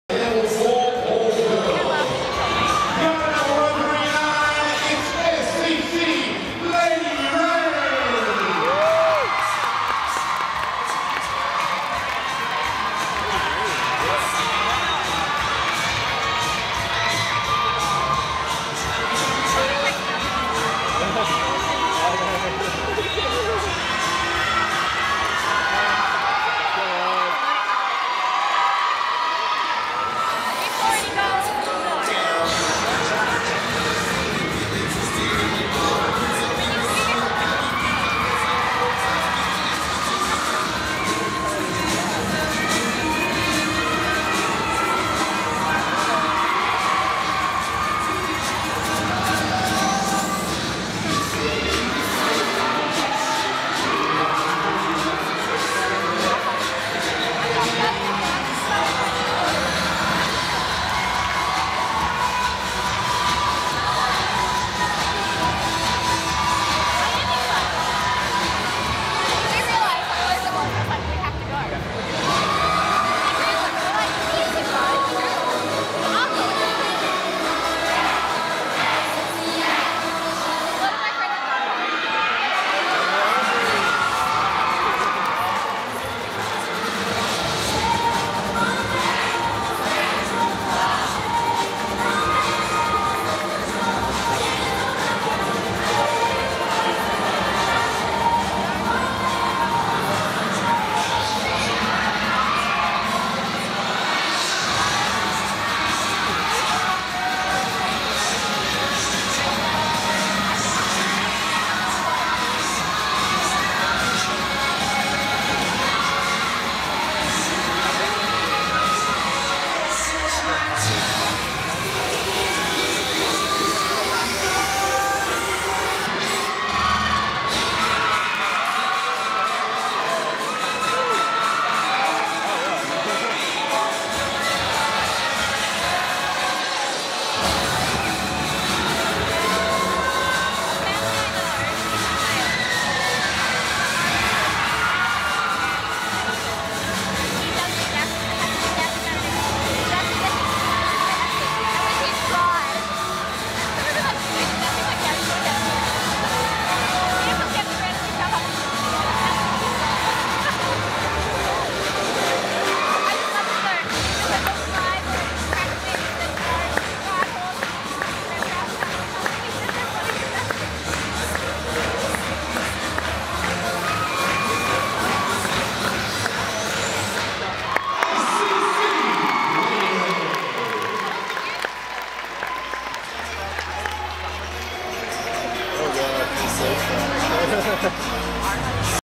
Southern Cross Cheerleading Lady Reign (level 4 all girl) - 2014 AASCF Winterfest
Open level 4 all girl